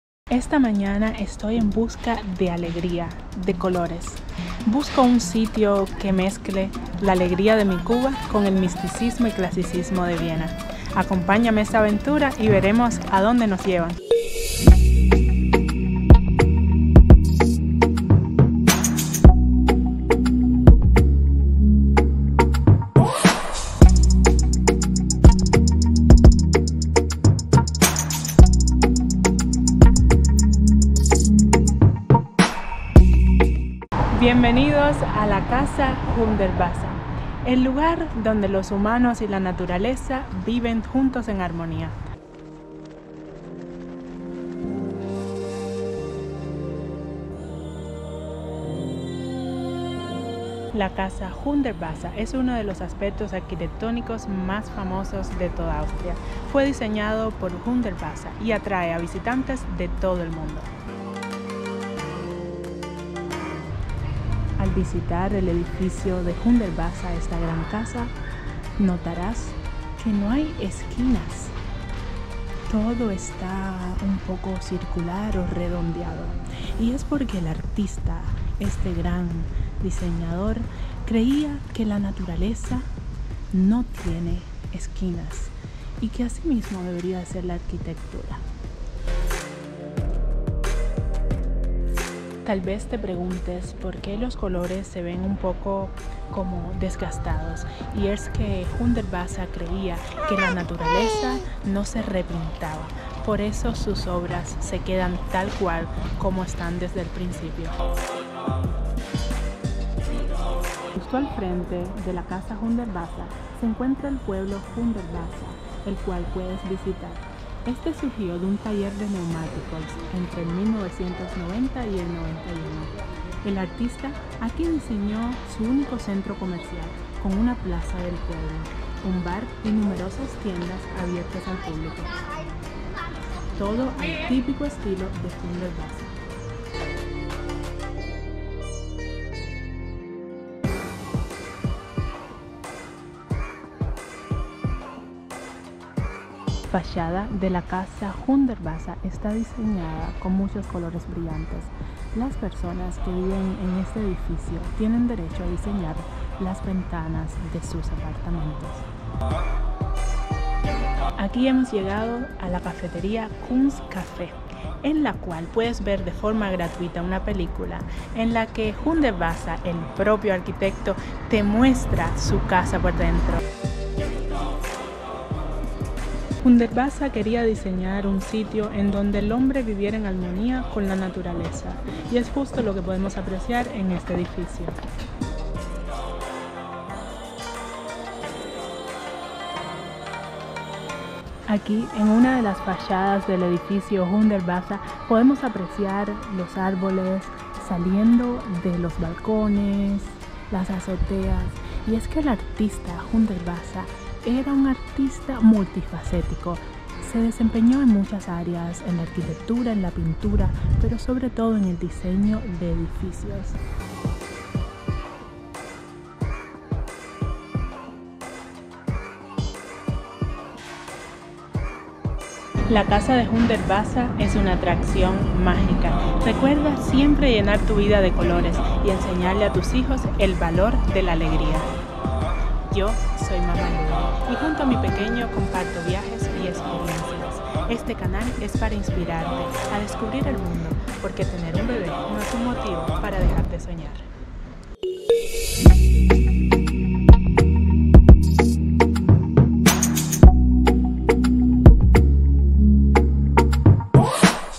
Cuba en Viena! Visita La Hundertwasser House en Viena cuando extrañes La Habana, Cuba.
La casa Hundertwasser en Viena es uno de los aspectos arquitectónicos más destacados de Austria. Fue diseñada por Friedensreich Hundertwasser y atrae a visitantes de todo el mundo. Acompáñame y descubramos juntos esta maravilla arquitectónica.